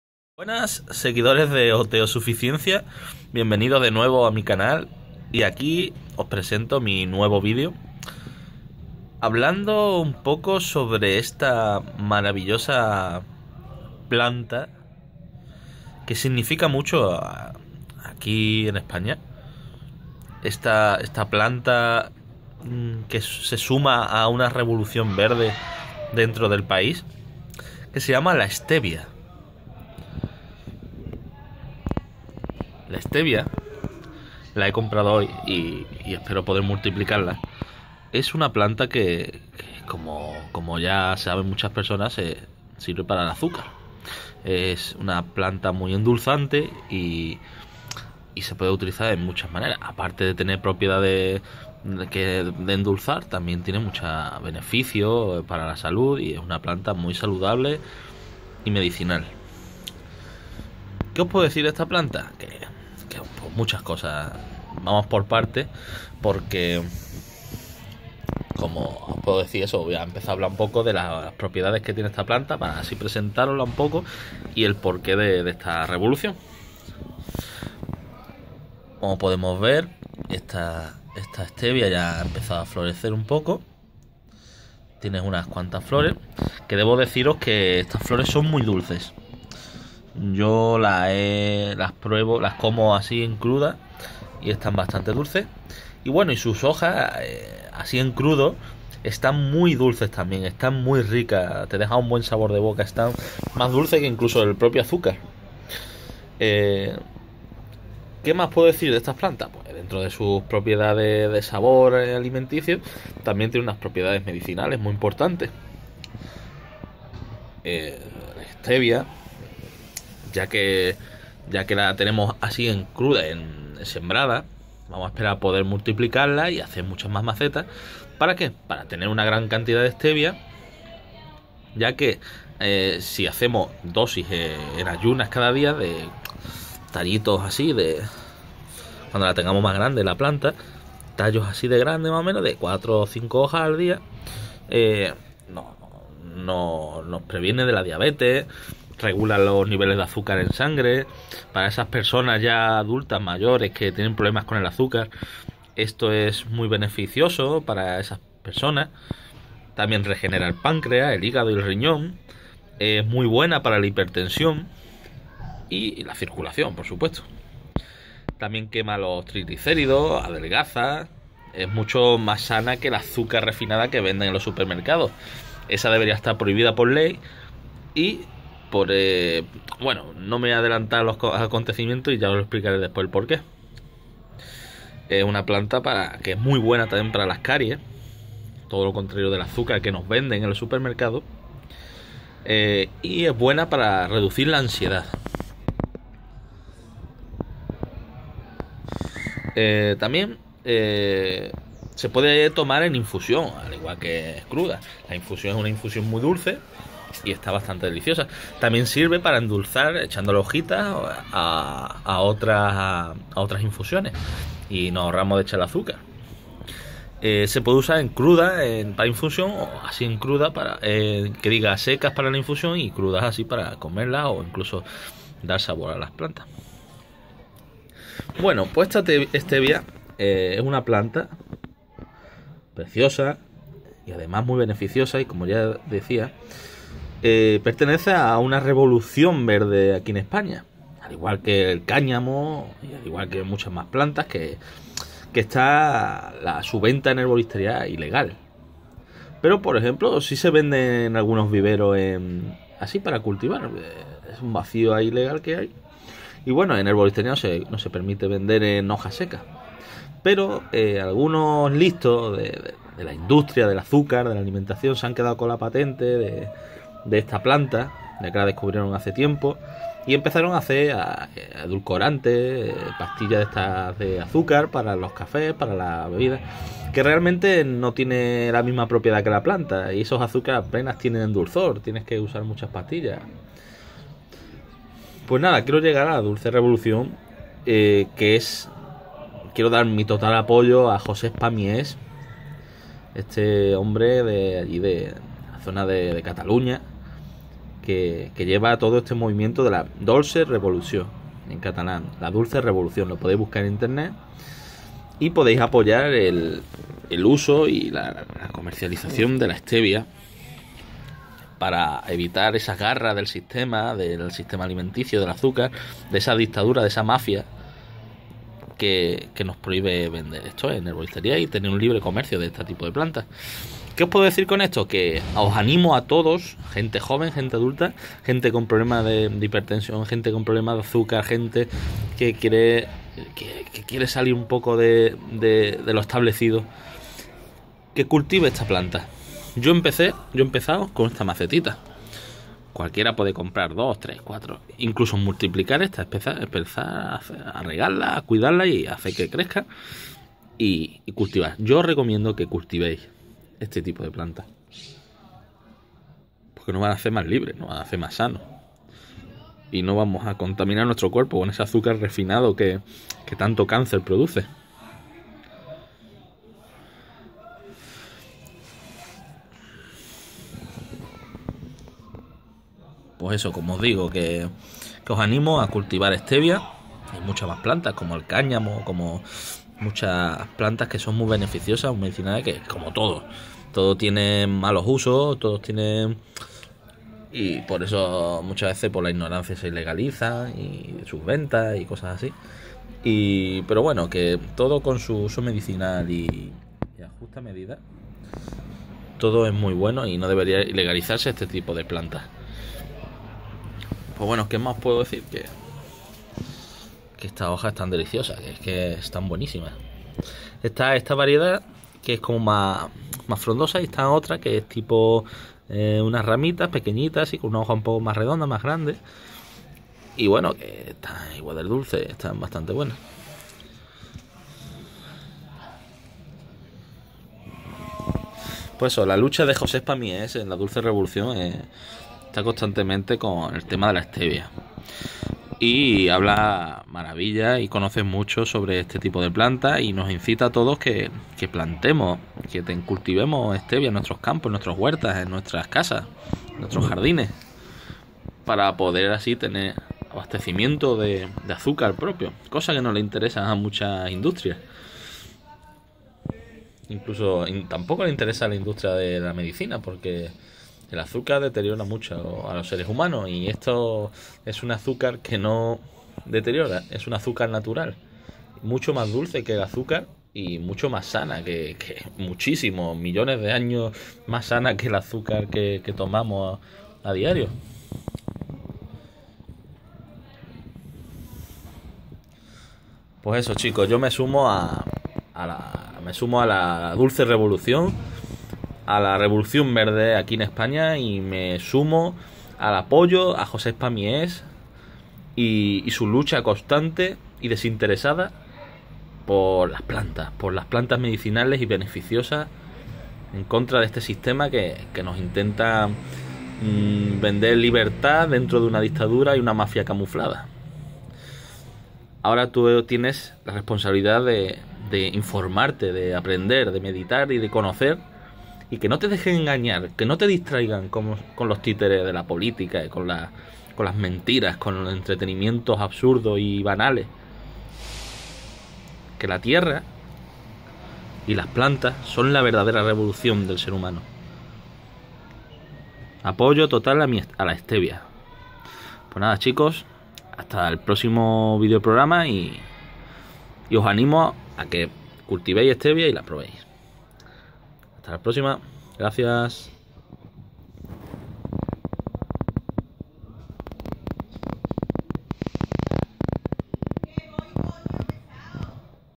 Stevia "La dulce revolución"/ OteoSuficiencia.
Hoy hablamos un poco sobre la Stevia Rebaudiana. Mi total apoyo a la dulce revolución y a Josep Pàmies. Dolça Revolució. Un edulcorante natural y sano de verdad.